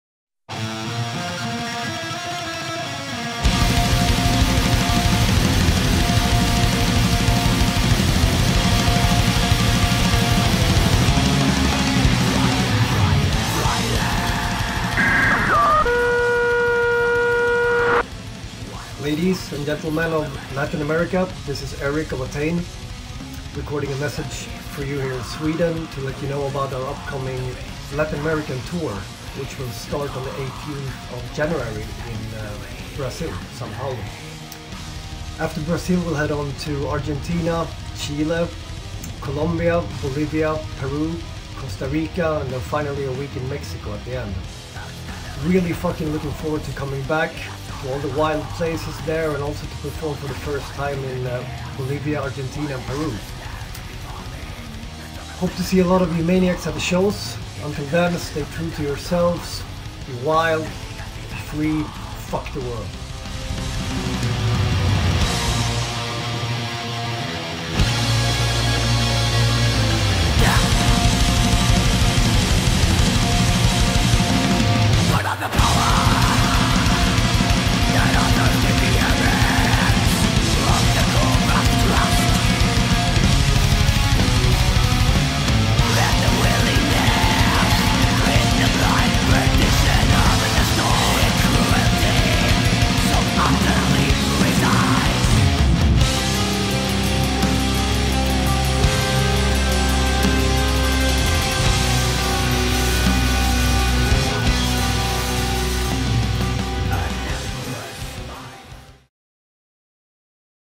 Watain - Erik Danielsson Call Off (Furor Diabolicus Latin America)
Video Editing, Art Direction, Subtitles and Production by 3468 Creations (Rezioth Videos are now 3468 Creations).

This is a call off video made to Talent Nation Productions for
Watain on the“Furor Diabolicus Latin America Tour 2019”.

Posted By Rezioth and Absiel from 3468 Creations.